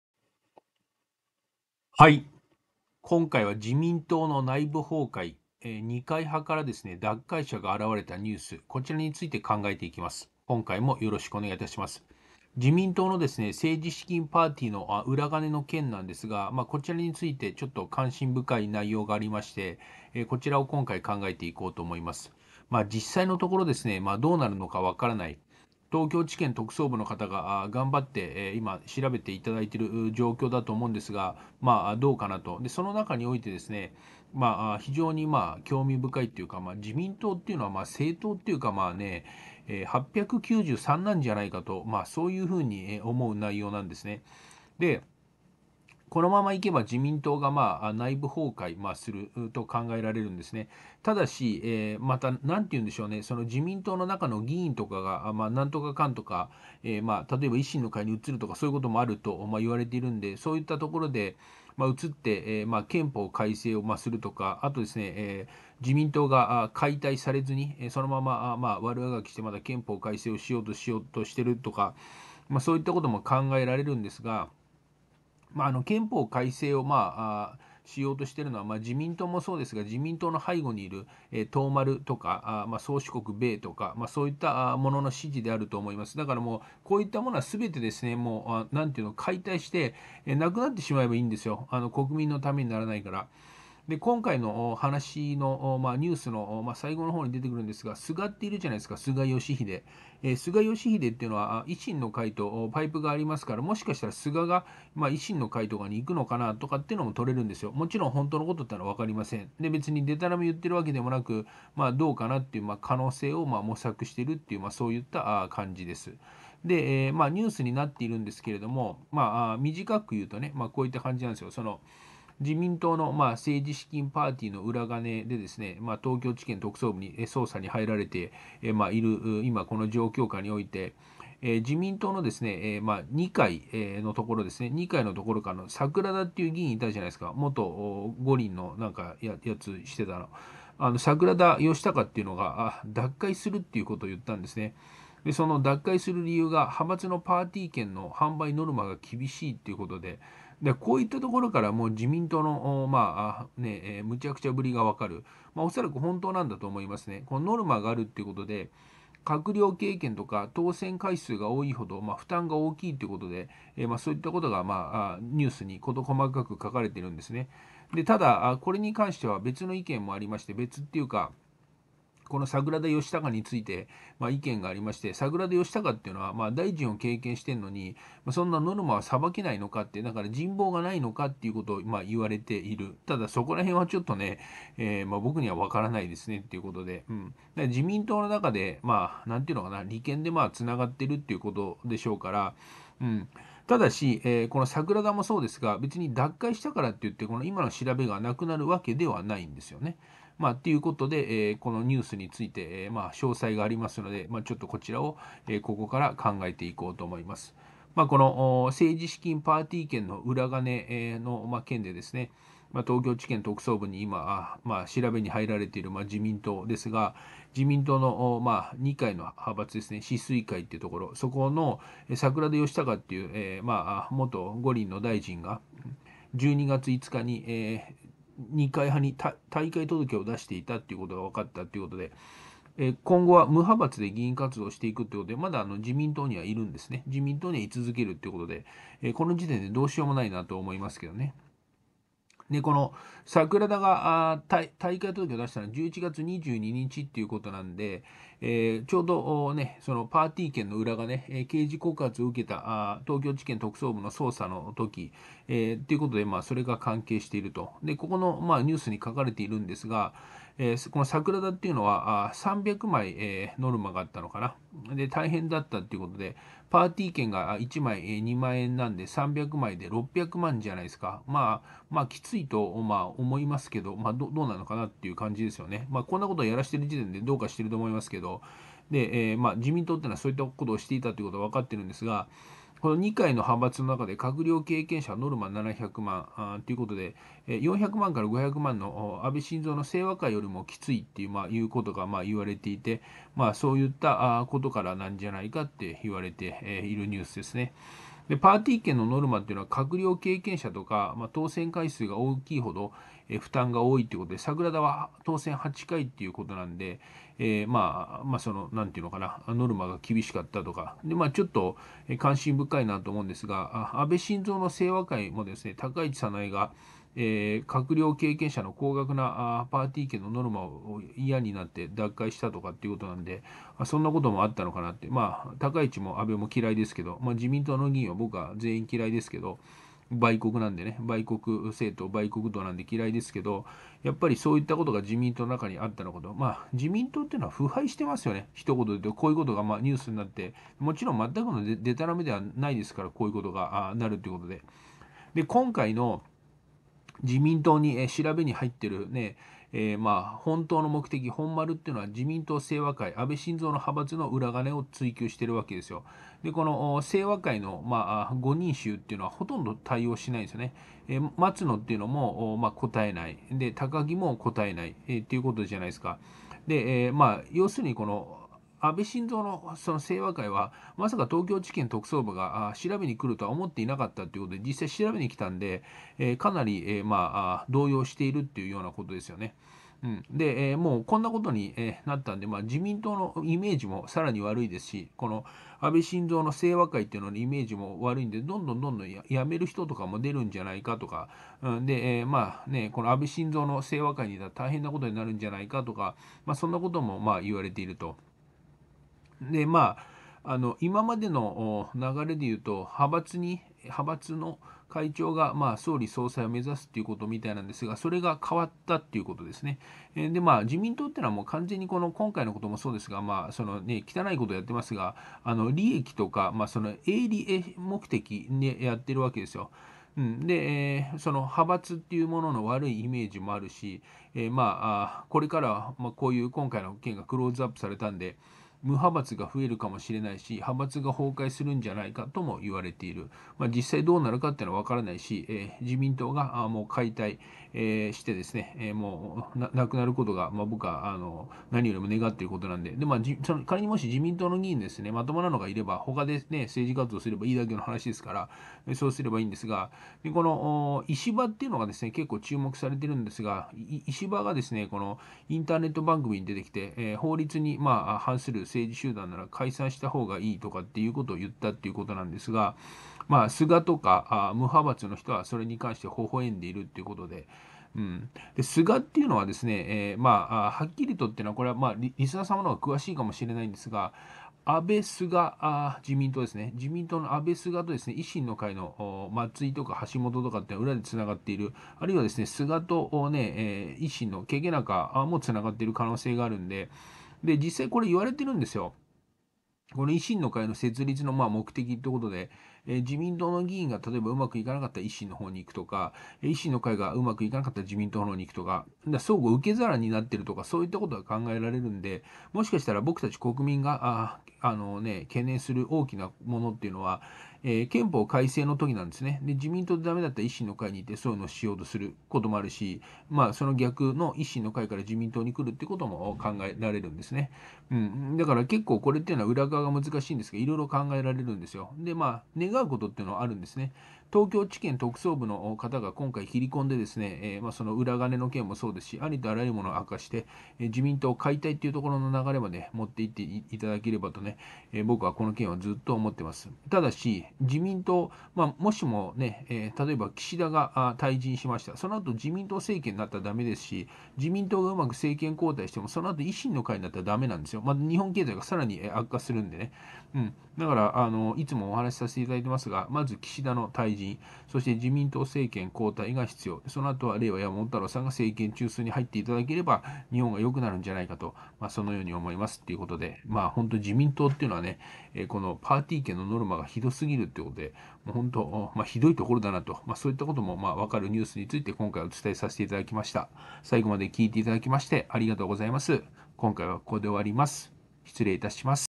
【自民党の内部崩壊】二階派から脱会者が現れたニュースについて。
今回は二階派から脱会者が現れたニュースについてお伝えしていきますので最後まで聞いていただければと思います。